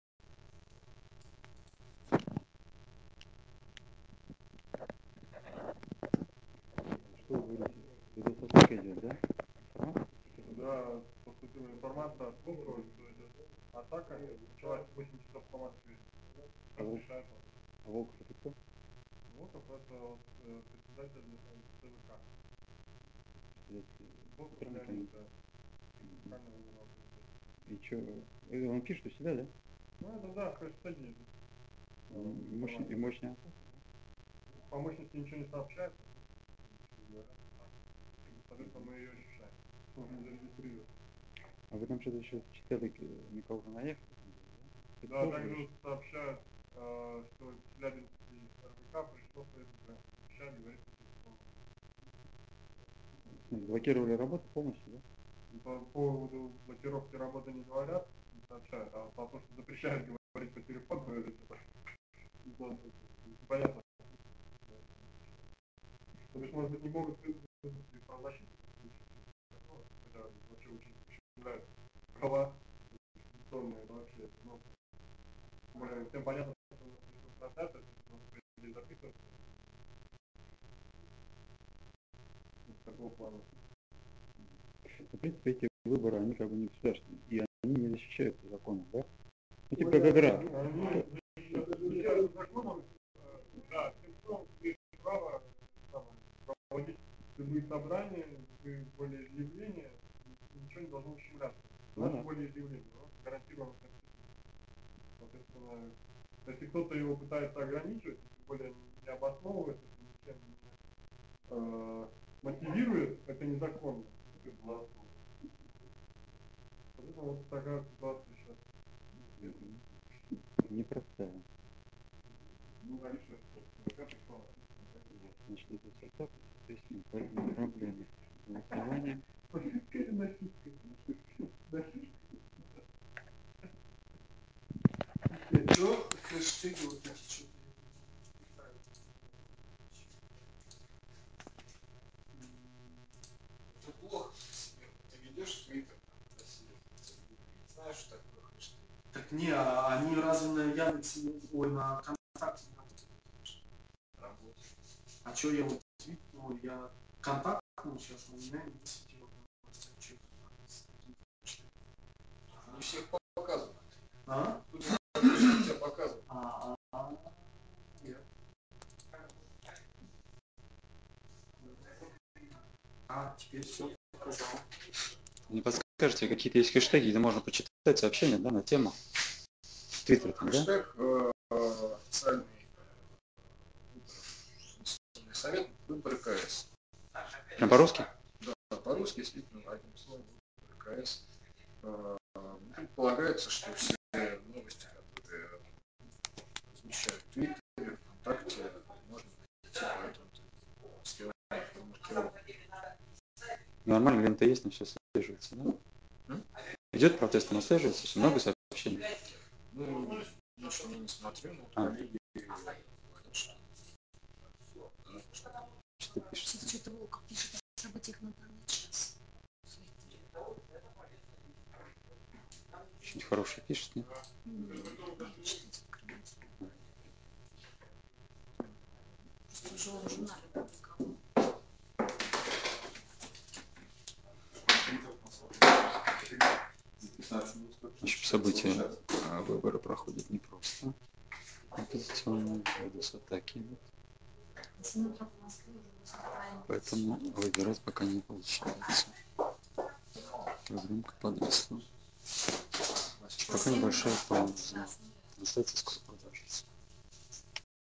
08 Выборы в КС оппозиции. Петербург 12.10.20.
В Петербурге, как и по всей стране прошли выборы в Кординационный Совет оппозиции. Оператор TV-live провёл весь день на всех 3-х избирательных участках. В отличие от официальных выборах, никаких проблем со съёмкой и трансляцией с выборов не было. Выборы прошли без нарушений, демократично и свободно.

Council elections in Kordinatsionny Russian opposition. St.Petersburg.